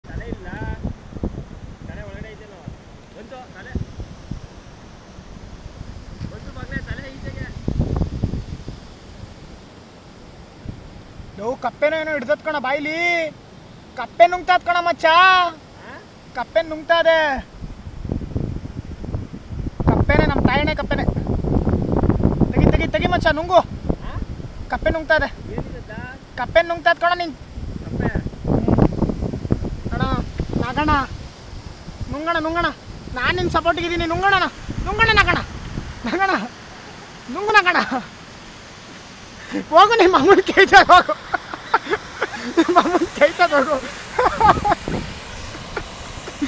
Black Cobra eating a Frog...
WARNING: The following contains graphic violence. This is a video of a cobra eating a frog near my home so i just seen that and recorded the video...